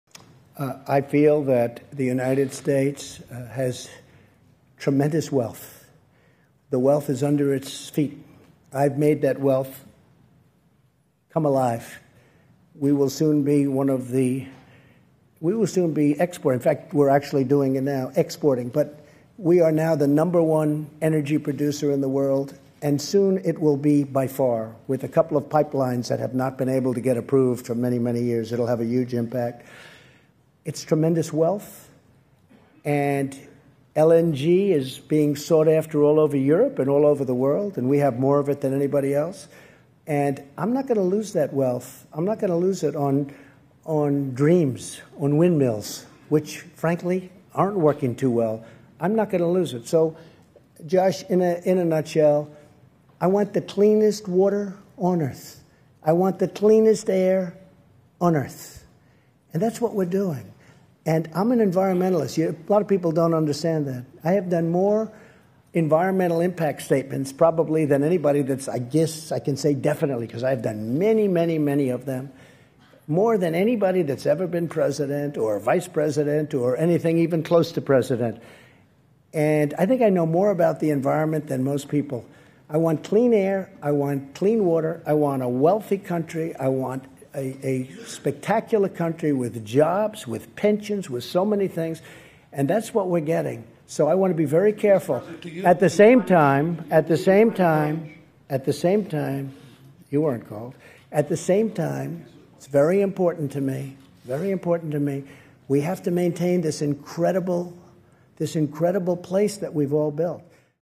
Trump Says Won't Lose U.S. Wealth To Dreams, Windmills
August 26, 2019 - President Donald Trump dismissed concerns about climate change at the G7 summit in France Monday, saying he's "not going to lose" America's wealth "on dreams" and "windmills." He skipped a session on the topic earlier in the day. (Aug. 26)